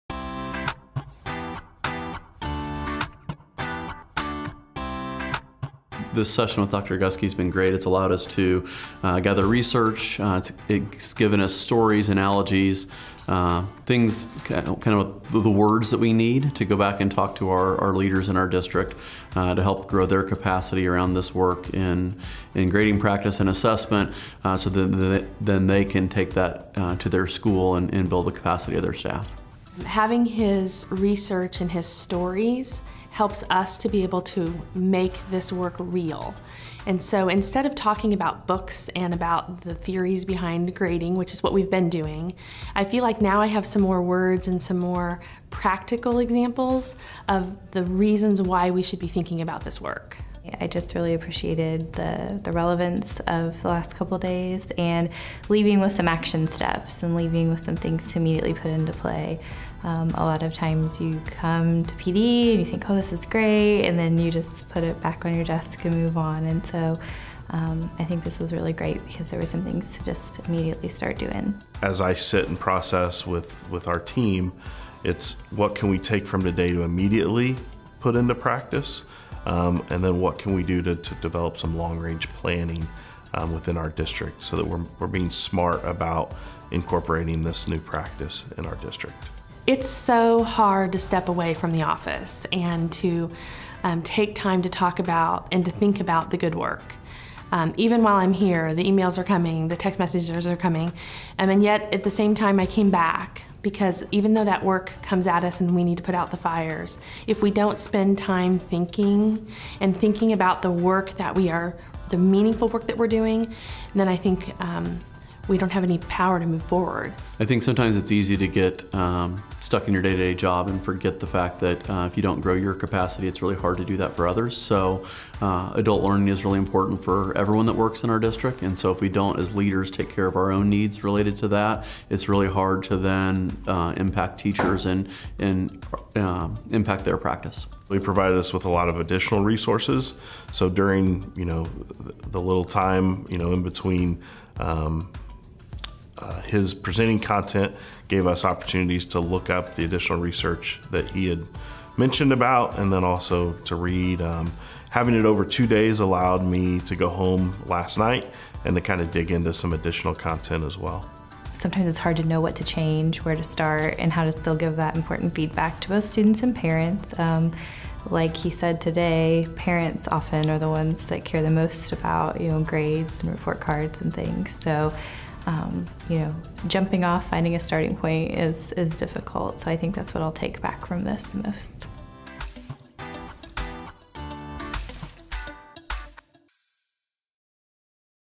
IN the LEAD - Strand 5 Competency Based Grading Participant Interviews
In this video, attendees share their experiences at the Strand 5 - Competency Based Grading with Dr. Tom Guskey sessions for the IN the LEAD workshops by Keep Indiana Learning